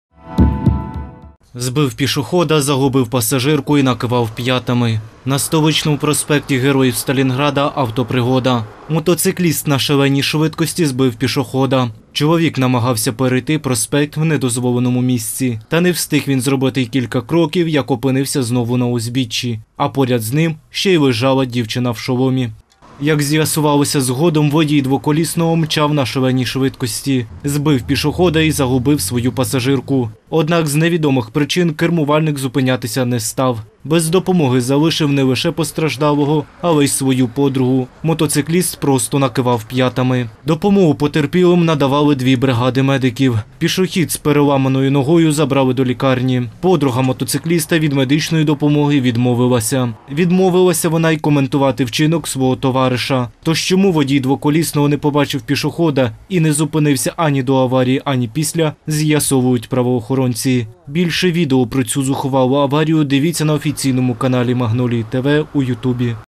14.05. У Києві Мотоцикліст Збив Пішохода І Втік.
14.05. У Києві Мотоцикліст Збив Пішохода І Втік.